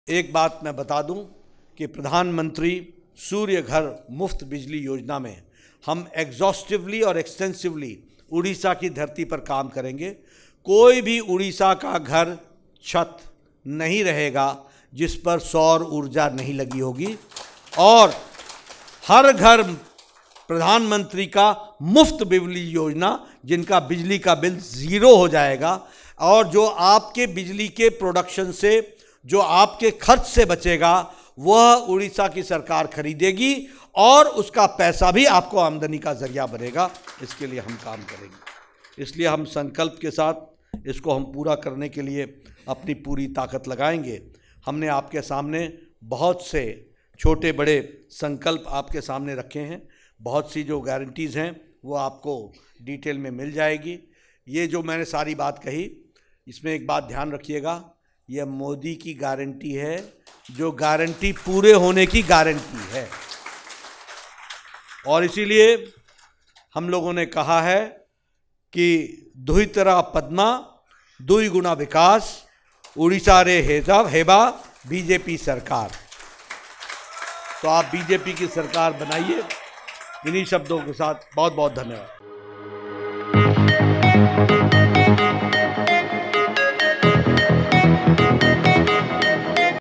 “PM-Surya Ghar: Muft Bijli Jojana across urban and rural households of Odisha“ | Odisha Reporter
BJP To implement the PM-Surya Ghar: Muft Bijli Jojana across urban and rural households of Odisha.
ପ୍ରଧାନମନ୍ତ୍ରୀ ସୂର୍ଯ୍ୟଘର ମାଗଣା ବିଜୁଳି ଯୋଜନା ମାଧ୍ୟମରେ ଓଡ଼ିଶାର ଘରଗୁଡ଼ିକୁ ବିଜୁଳି ଯୋଗାଇଦିଆଯିବ । ଲୋକଙ୍କ ବିଜୁଳି ବିଲ୍ ଜିରୋ ହୋଇଯିବ । ମୋଦିଙ୍କ ଗ୍ୟାରେଣ୍ଟି ମାନେ ପୂରଣ ହେବ ନିଶ୍ଚୟ: ଜେପି ନଡ୍ଡା, ବିଜେପି ରାଷ୍ଟ୍ରୀୟ ଅଧ୍ୟକ୍ଷ